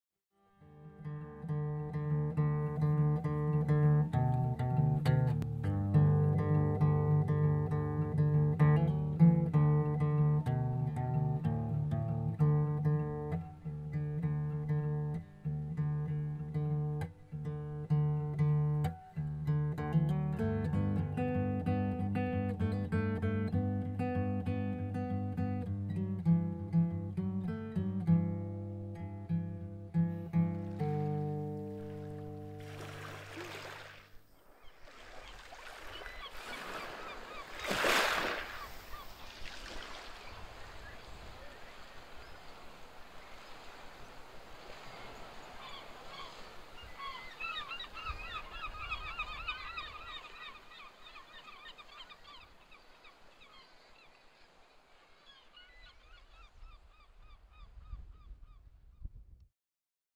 Take time… Find the sense! Forillon National Park
This video, which highlights the beauties of Forillon National Park through a visitor's five senses, is part of a series of six inviting visitors to "find the sense".
They inspire tranquility, wonder and rejuvenation. They are an invitation to take a time off to reconnect to nature and to one's self.